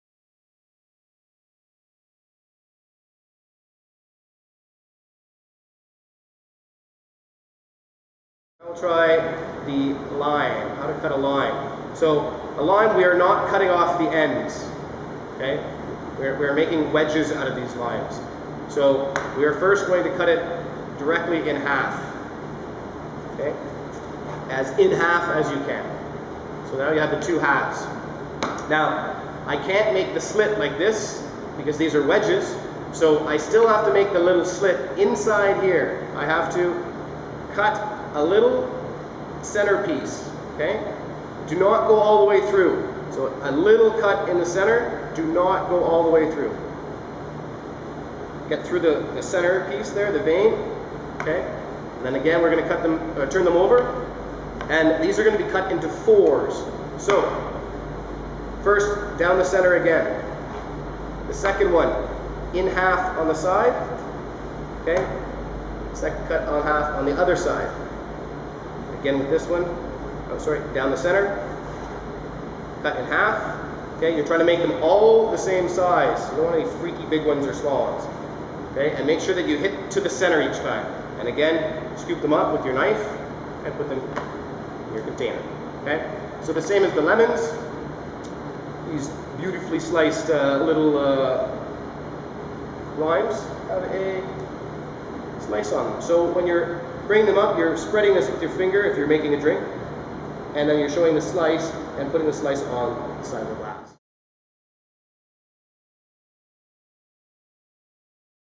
How to Cut Lime Wedges: Cutting Limes Bartending Tutorial
Learn how to cut lime wedges in this bartending basics video tutorial.  Cutting limes and lemons for the bar is not at all difficult but you'd be amazed how many bartenders do it incorrectly.

The main thing to keep in mind when learning how to cut a lime is that you MUST cut the little slits down the center of the lime wedges.  If you don't put a slit on each lime wedge, you won't be able to slip in on the rim of a glass when you're making mixed drinks and cocktails later.

How to slice a lime is different from how to slice a lemon.  Generally speaking, limes are cut into wedges and lemons are cut into slices (due in part because of the size difference between the limes and lemons); however you can cut a lime into slices, wedges or full rings.

How to Cut Lime Wedges:  Bartending Basics Bar tending Tutorial